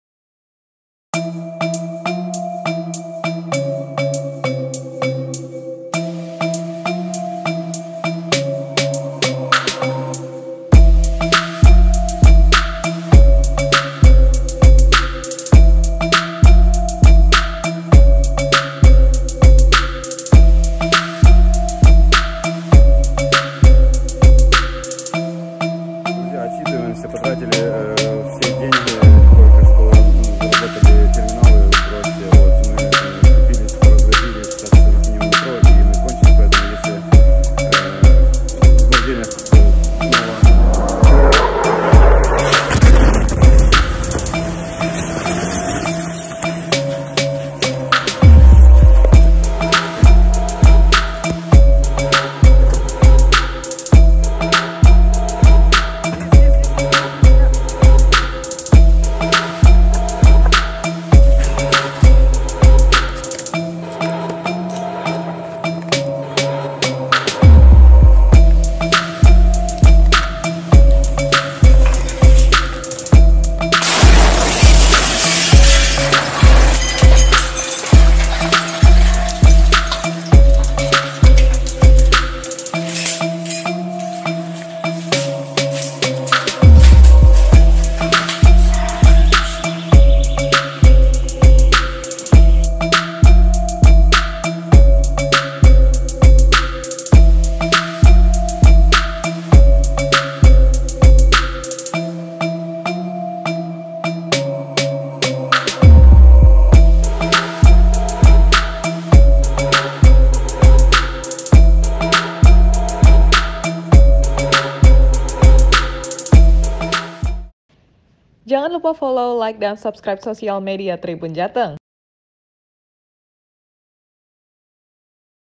Lagi Asik Ngevlog, Pria Ukraina Nyaris Terkena Rudal Rusia
Invasi Rusia ke Ukraina benar-benar membuat negara berbendara biru-kuning tersebut tak berkutik.

Bahkan warga sipil juga menjadi korban akibat invasi yang masih berlangsung hingga saat ini.

Salah satunya seorang vlogger asal Ukraina bernama Nikita Demenkov, ia nyaris tewas saat mengabadikan kondisi Kota Kharkiv.